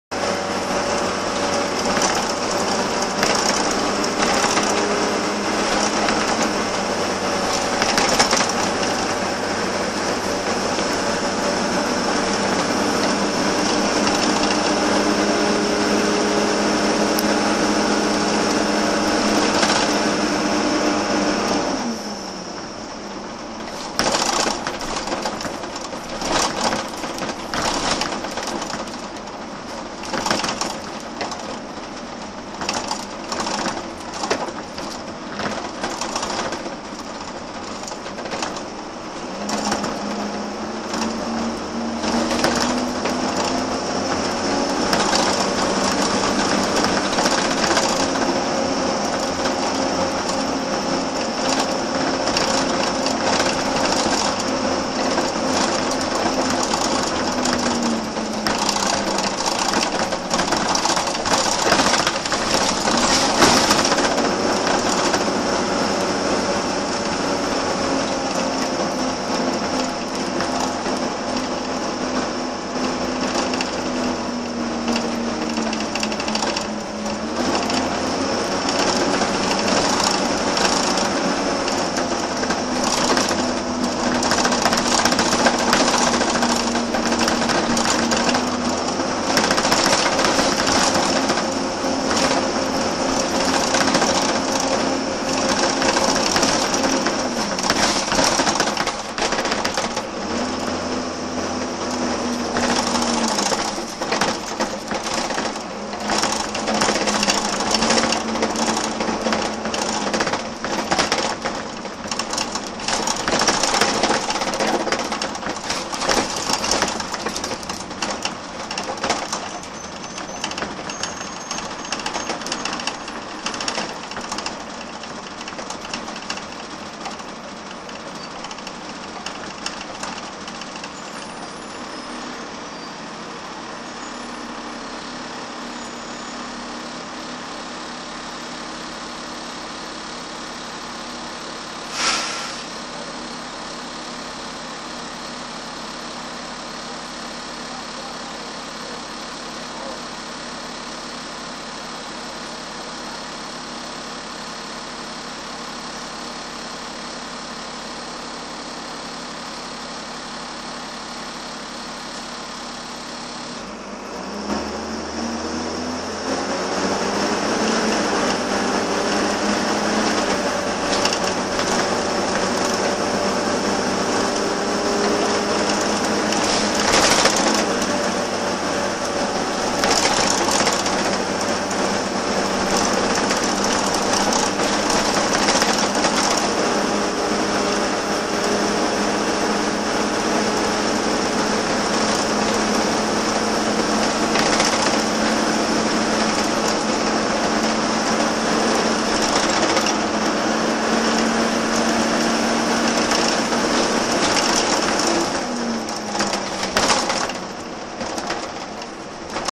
First West of England 42731 T731 REU
Here we see a journey on Dennis Dart SLF 42731 T731 REU seen on route X75 to Bridgwater.